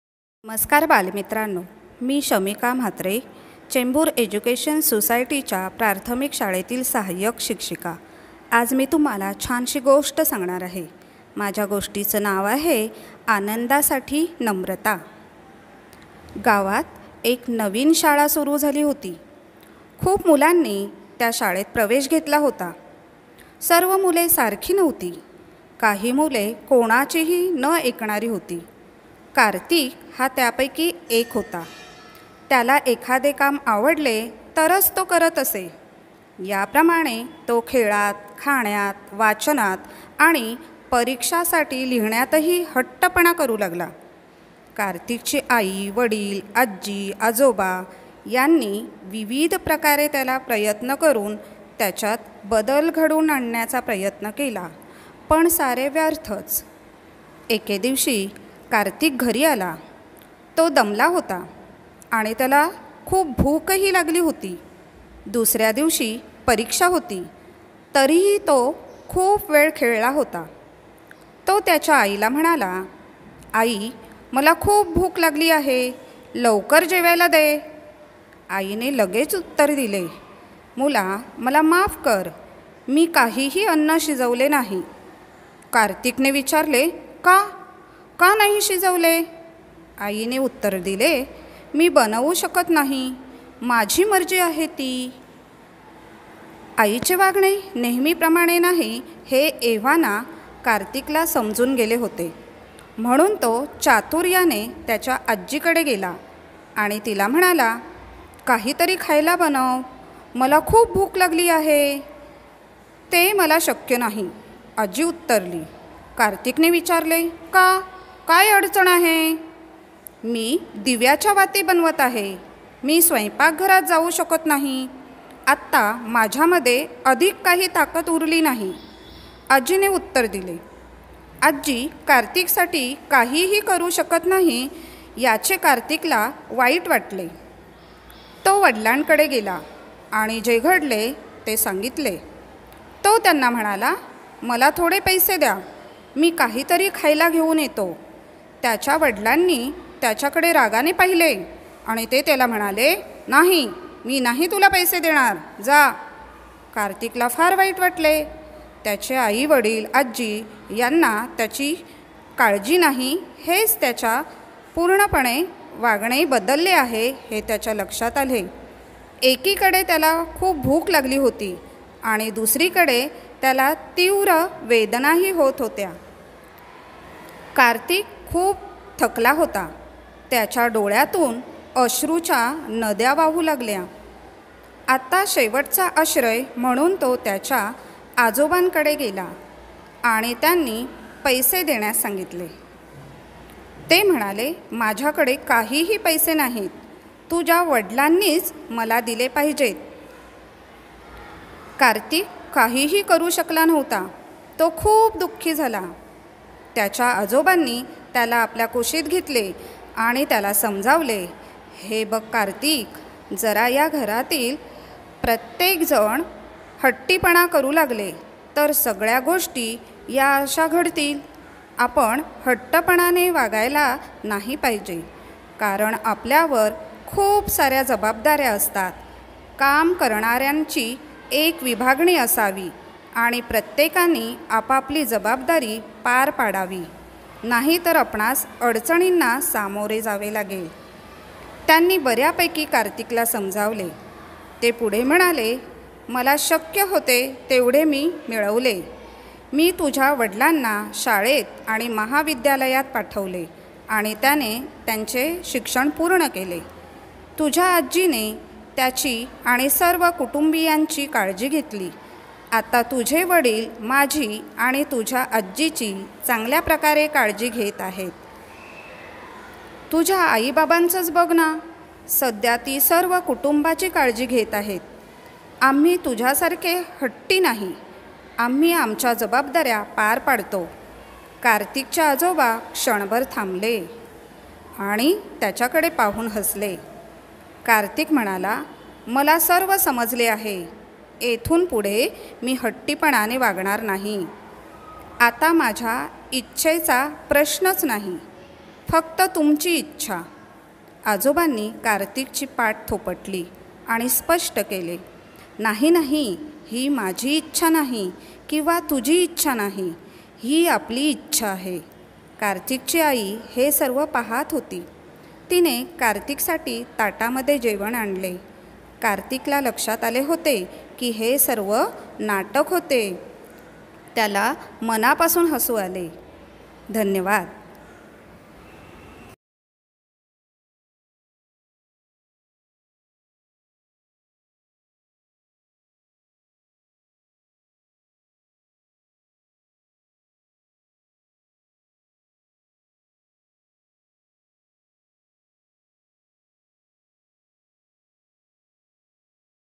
आनंदासाठी नम्रता वाचू आनंदे अभिवाचन शमिका म्हात्रे चेंबूर एज्युकेशन सोसायटीची प्राथमिक शाळा
चेंबूर एज्युकेशन सोसायटीची प्राथमिक शाळा
आनंदासाठी नम्रता वाचू आनंदे अभिवाचन शमिका म्हात्रे